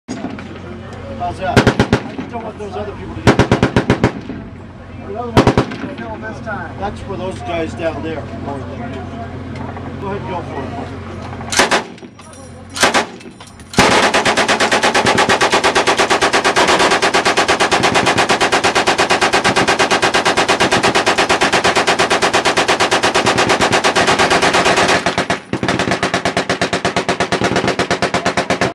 Machine Gun Shoot M60 Fernley NV 2006
My friend Roy tries his skills with the M60 at the Machine Gun Shoot in Fernley NV 2006.